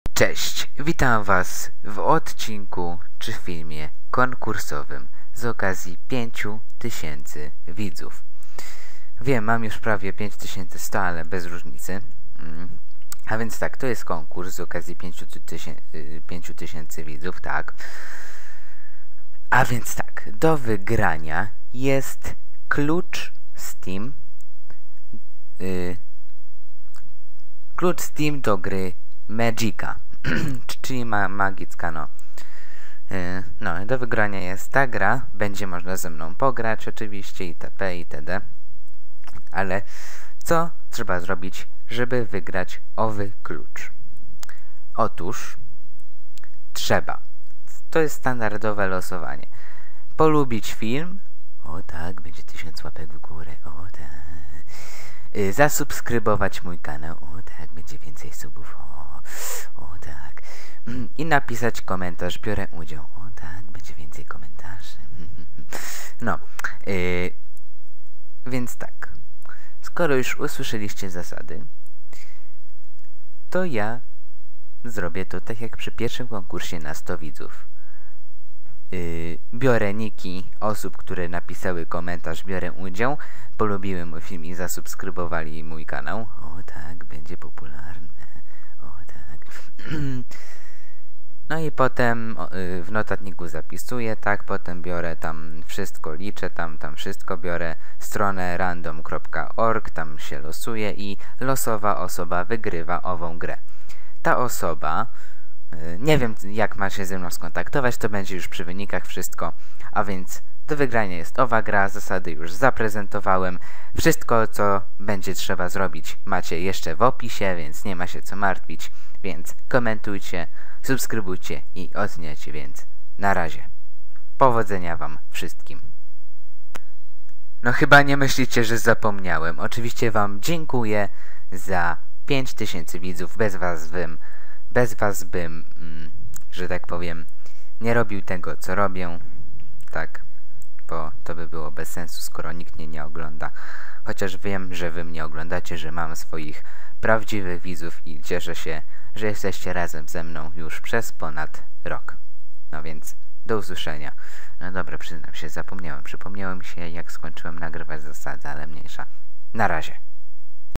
Konkurs z Okazji 5,000 widzów! Do wygrania Klucz Steam do gry ''Magicka'' (Zakończony)
Aby wziąć udział w konkursie musisz:
1. Dać łapkę w górę
2. Subnąć mój kanał
3. Napisać komentarz ''biorę udział

Konkurs Trwa do 20 Stycznia 2013r. Kilka dni później są wyniki. Powodzenia!